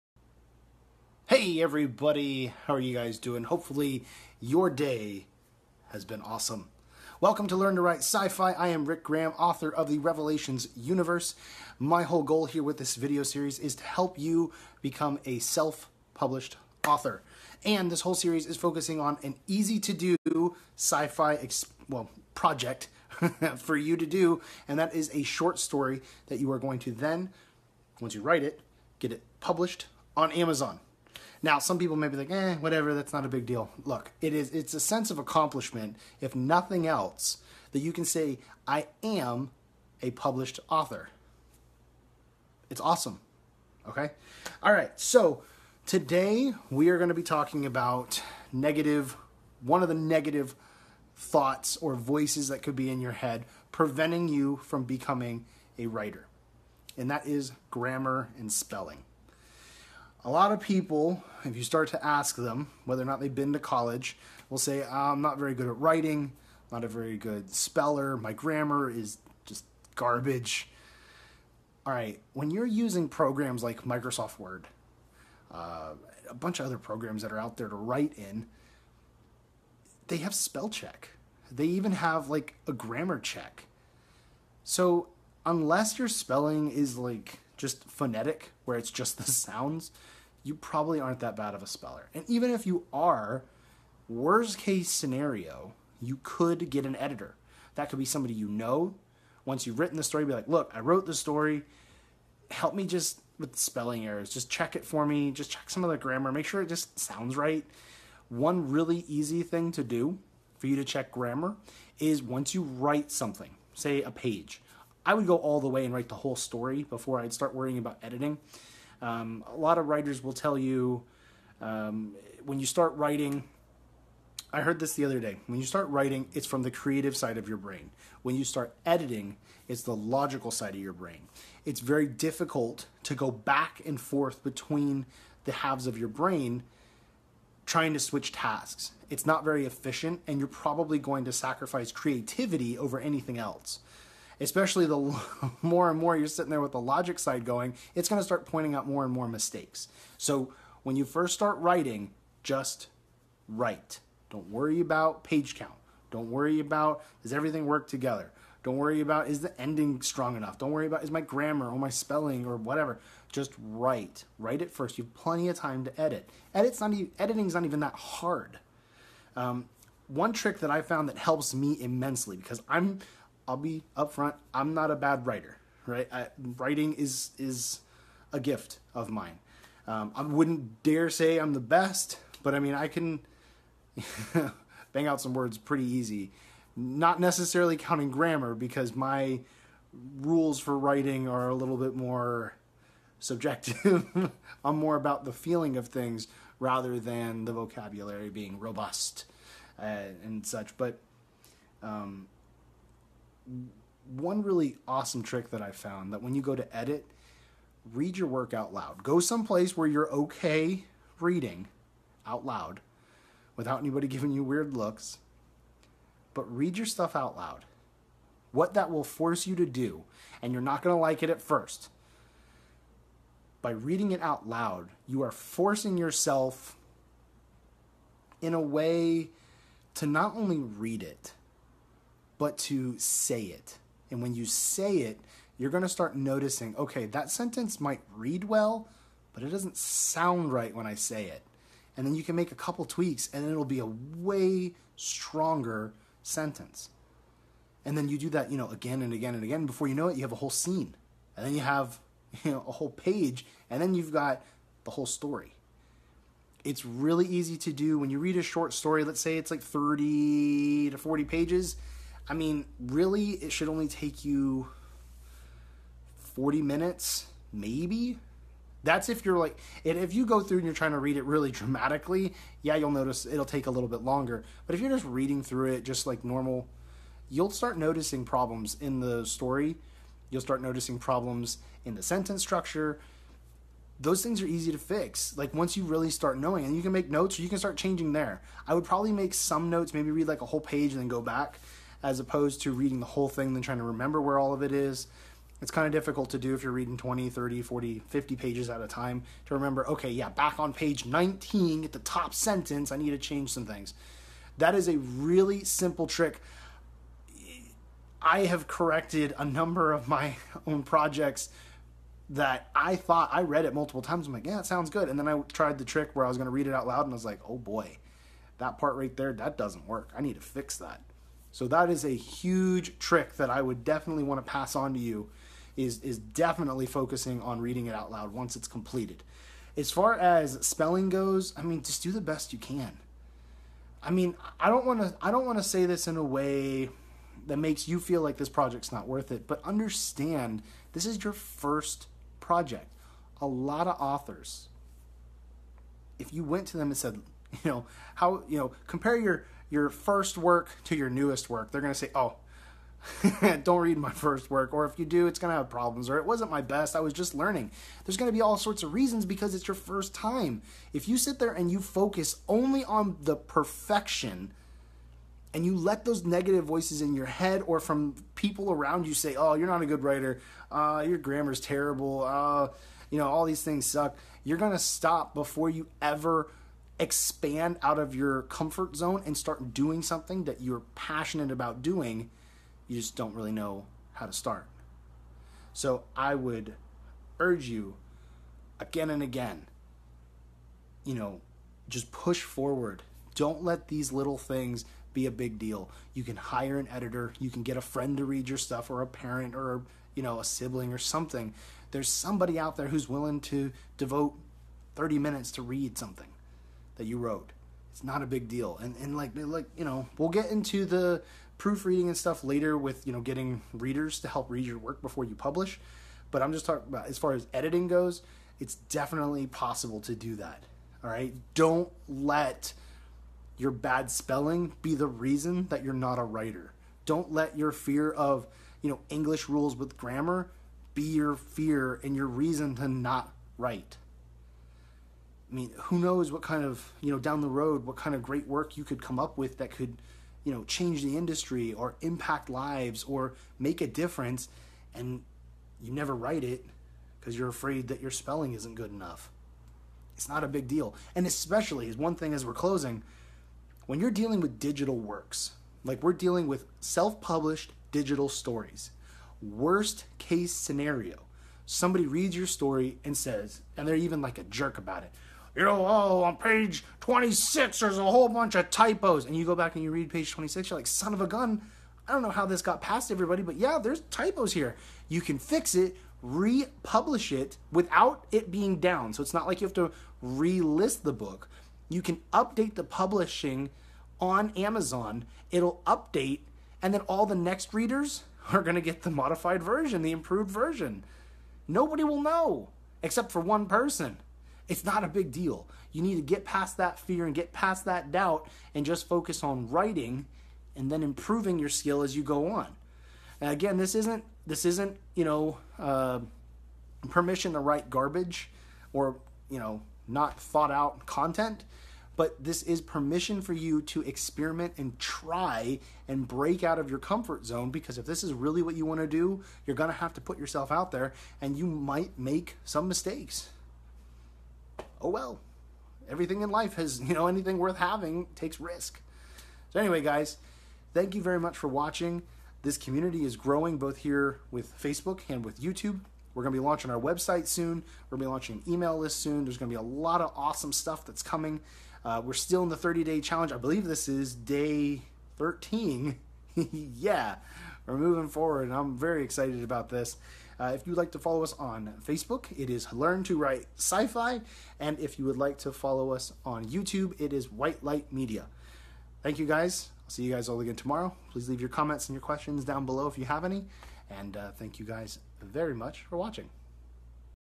Scared Of Making A Mistake As A Writer? WATCH THIS!! - Learn To Write SciFi - Episode 12
In this series I am going to teach you how to become a self-published author. Tens of millions of Americans have an idea for a book. Are you one of them?